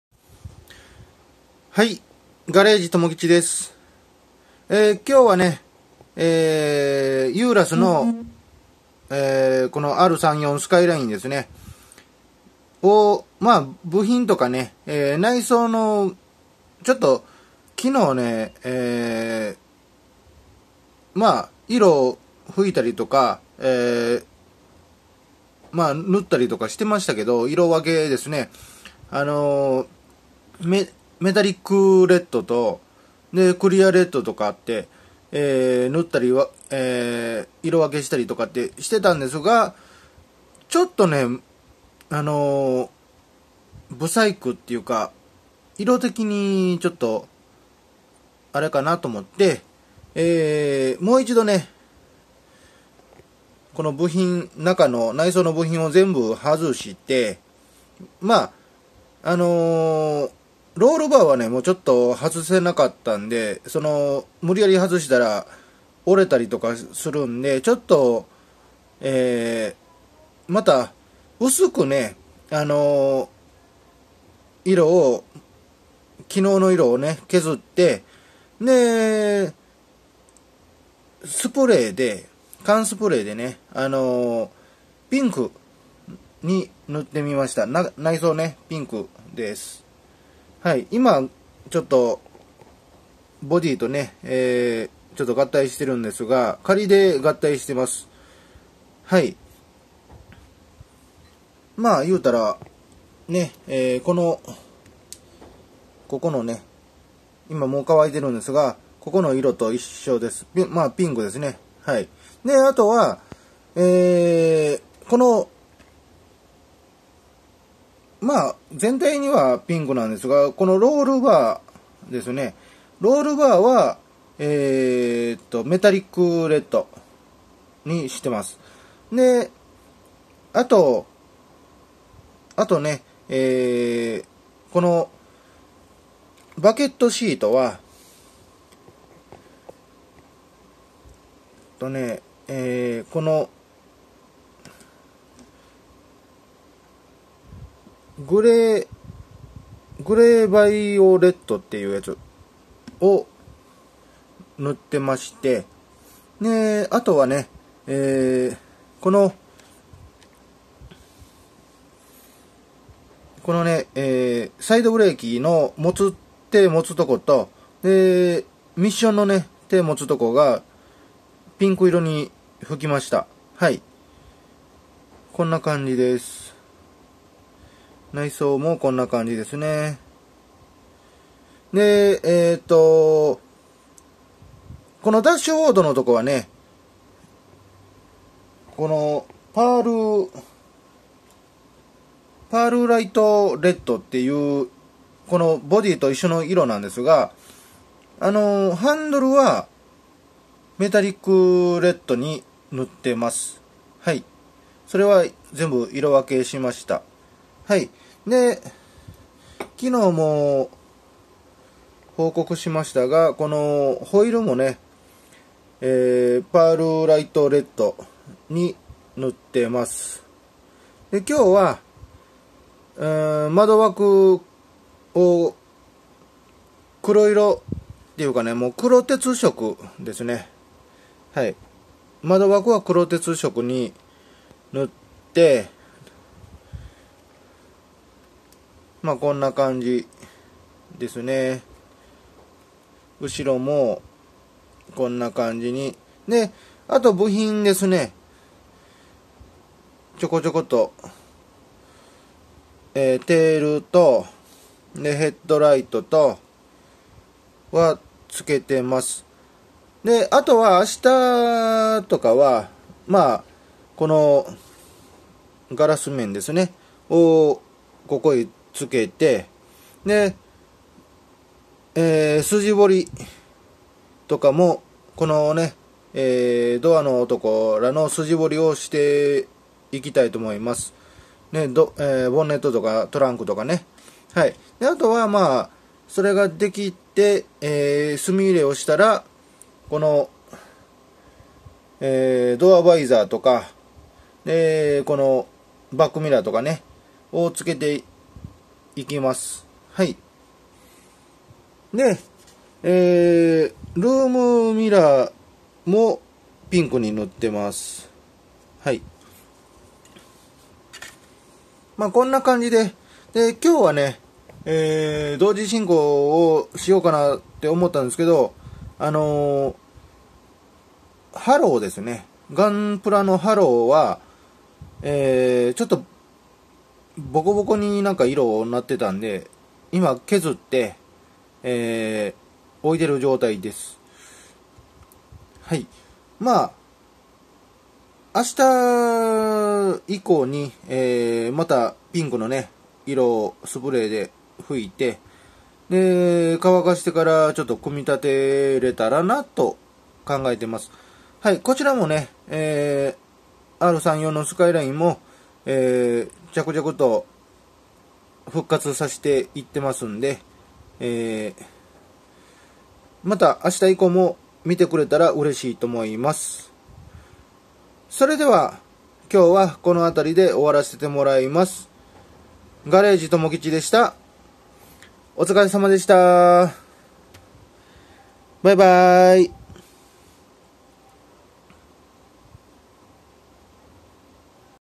ユーラスＲ３４スカイラインの色吹き直しが今日はここまでできました😃👍(笑)
ユーラスＲ３４スカイラインのやり直しをして色も変えてまどわくも塗って仮にボディーとシャーシを合体してみました😃部品もちょっとつけたよ🙆(笑)チャンネル登録、コメント、グッドボタンをお願いします🙇⤵️